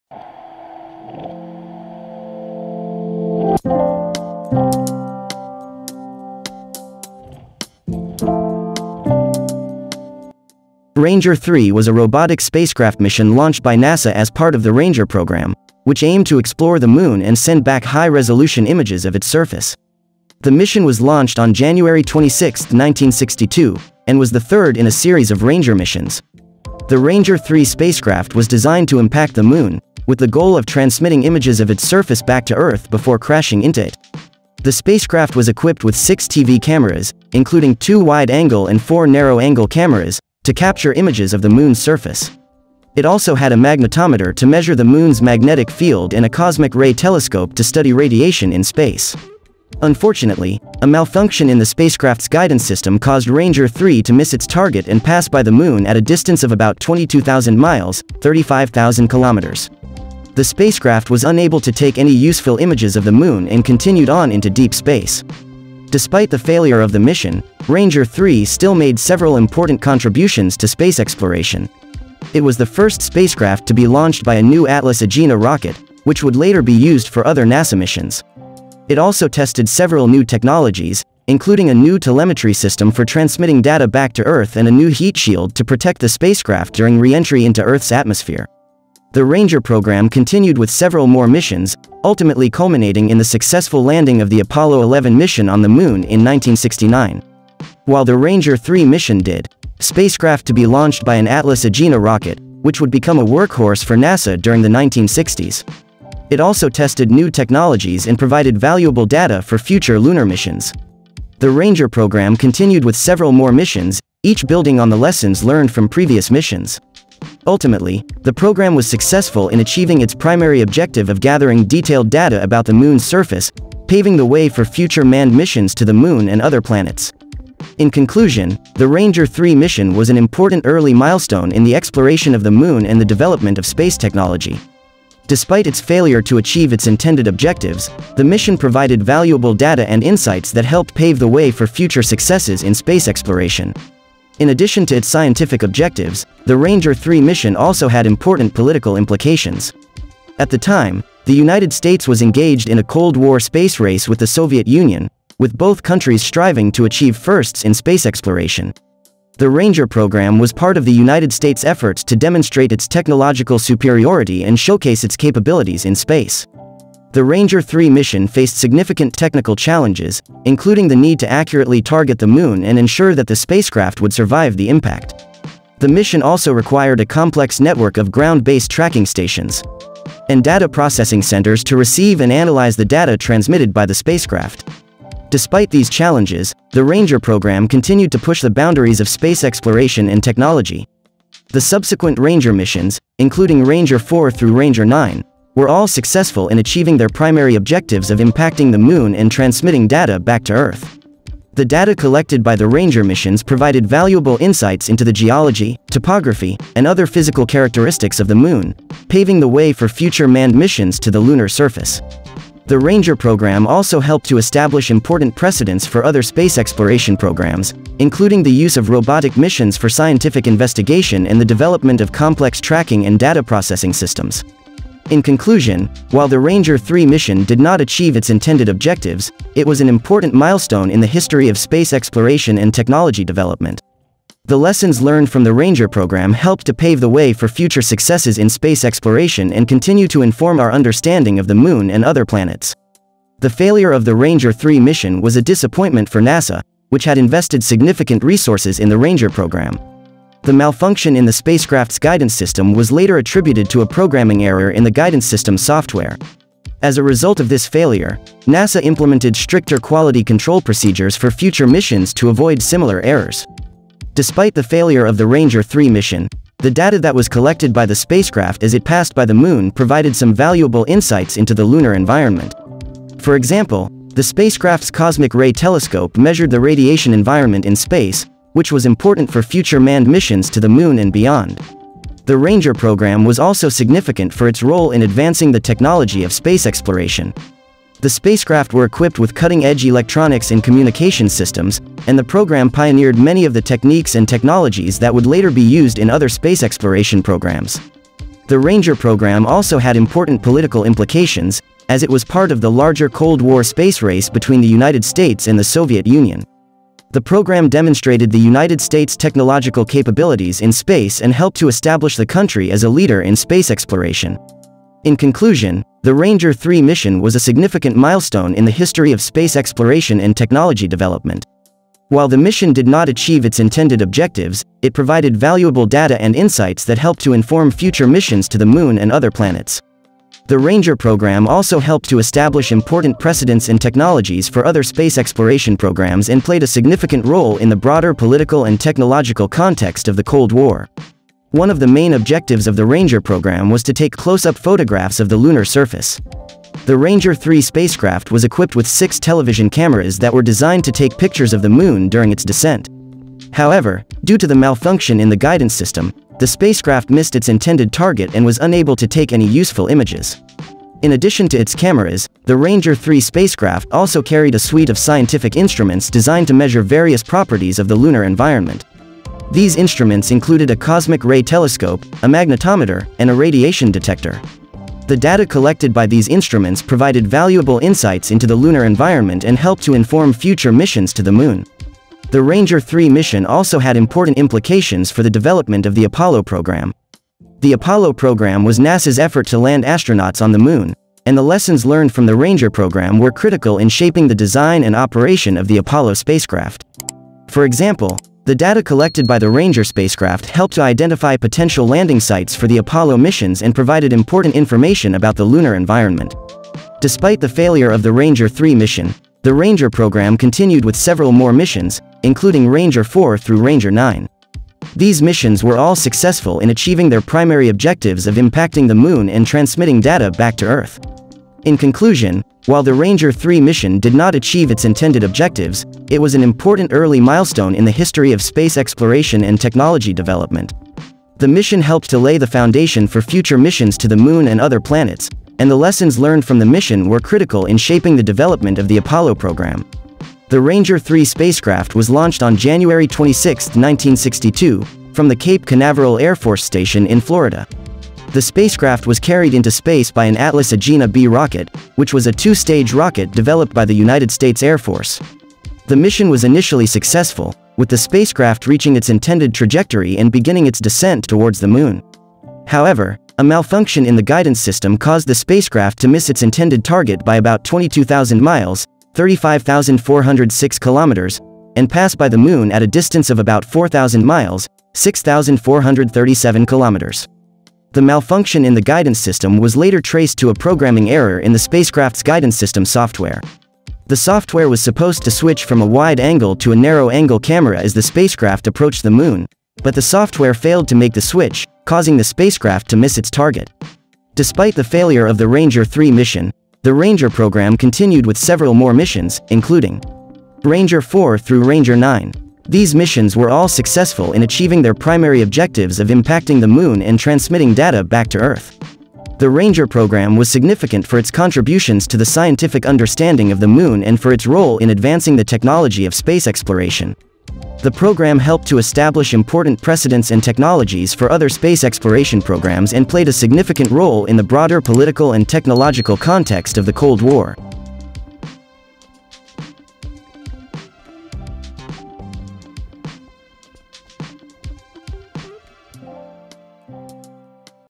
Ranger 3 Mission The Failed Lunar Landing That Paved the Way for Future Success
Ranger 3 was a robotic spacecraft mission launched by NASA as part of the Ranger program, which aimed to explore the Moon and send back high-resolution images of its surface.

The mission was launched on January 26, 1962, and was the third in a series of Ranger missions.

The Ranger 3 spacecraft was designed to impact the Moon, with the goal of transmitting images of its surface back to Earth before crashing into it.

The spacecraft was equipped with six TV cameras, including two wide-angle and four narrow-angle cameras, to capture images of the Moon's surface.

It also had a magnetometer to measure the Moon's magnetic field and a cosmic ray telescope to study radiation in space.

A malfunction in the spacecraft's guidance system caused Ranger 3 to miss its target and pass by the Moon at a distance of about 22,000 miles.

The spacecraft was unable to take any useful images of the Moon and continued on into deep space.

Despite the failure of the mission, Ranger 3 still made several important contributions to space exploration.

It was the first spacecraft to be launched by a new Atlas-Agena rocket, which would later be used for other NASA missions.

It also tested several new technologies, including a new telemetry system for transmitting data back to Earth and a new heat shield to protect the spacecraft during re-entry into Earth's atmosphere.

The Ranger program continued with several more missions, ultimately culminating in the successful landing of the Apollo 11 mission on the Moon in 1969.

While the Ranger 3 mission did not achieve its intended goal, it helped pave the way for future lunar exploration and provided valuable lessons for future space missions.

The Ranger program was initiated in 1959 by NASA as a series of robotic missions to explore the Moon and conduct scientific investigations.

The primary goal of the program was to gather detailed data about the Moon's surface, including its topography, composition, and other physical characteristics.

The spacecraft had a mass of 735 kilograms and was 3.1 meters tall.

The mission plan called for Ranger 3 to travel to the Moon and impact its surface, transmitting images of its journey and the lunar surface back to Earth before crashing into the Moon.

The spacecraft was designed to withstand the impact and continue transmitting data until it was destroyed.

Shortly after launch, a malfunction occurred in the spacecraft's guidance system, which caused it to veer off course and miss its intended target on the Moon by about 36,800 kilometers.

As a result, the spacecraft did not achieve its primary scientific objectives and was unable to take any useful images of the Moon's surface.